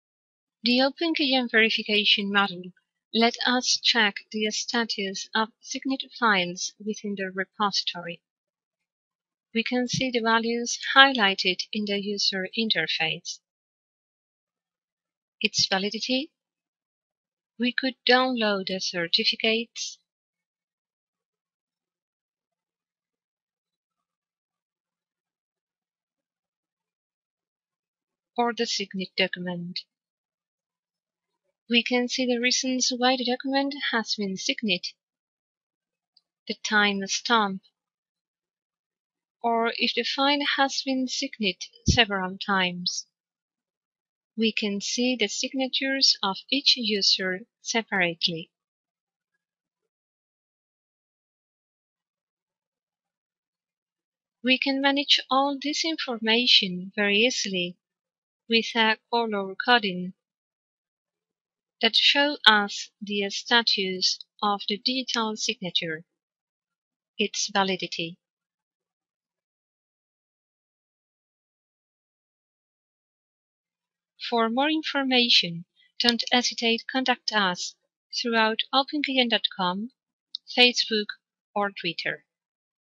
OpenKM - Verification digital signature tap
In this video, you can see the information about digitally signed files, available from the desktop area.